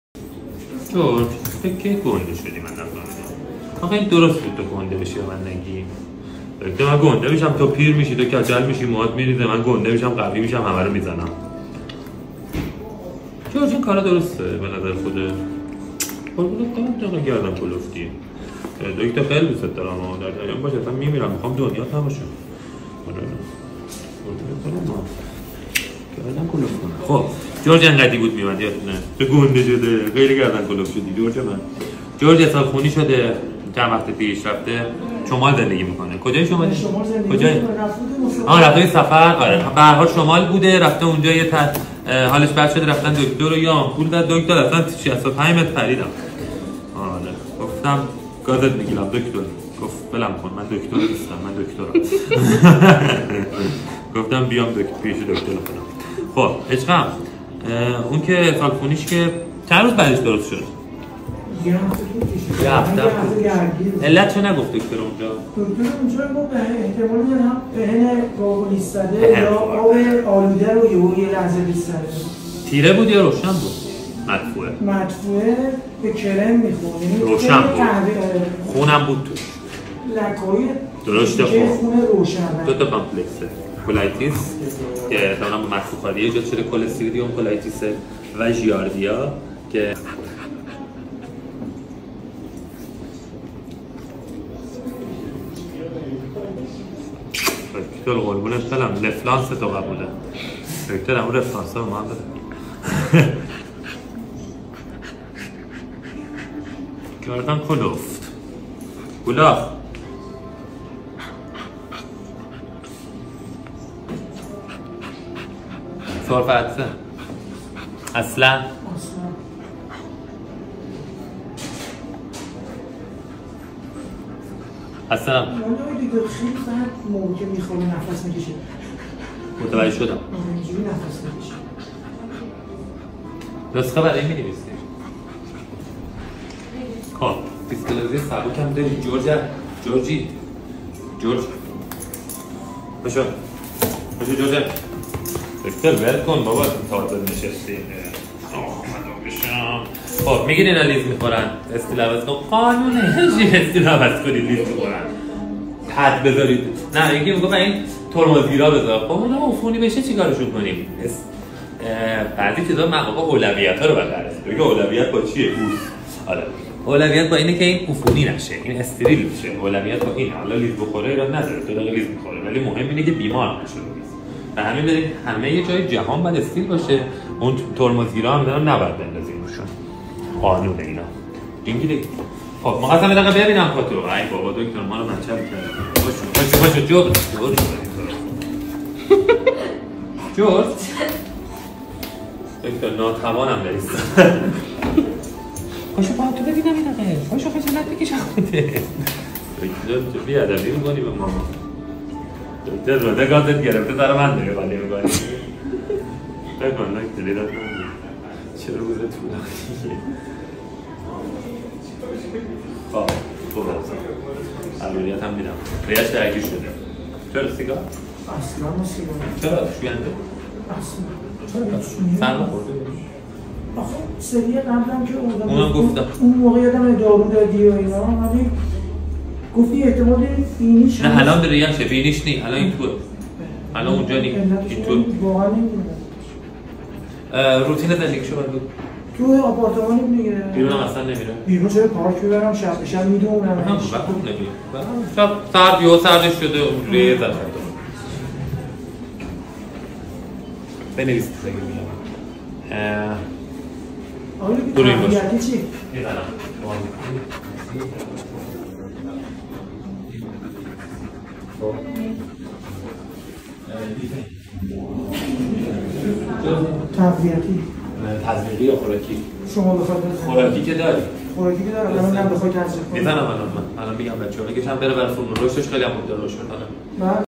ملاقاتی سه باره با جورجی عزیزم .
رفته شمال و اونجا دچار بیماری روده شده بوده ممنونم از پزشک حاذقی که سریعا تحت درمان گذاشتنشون ( متاسفانه اسمشون خاطرشون نبود ) .

(( حیوانات خانگی بهتریت دوستان بشریت))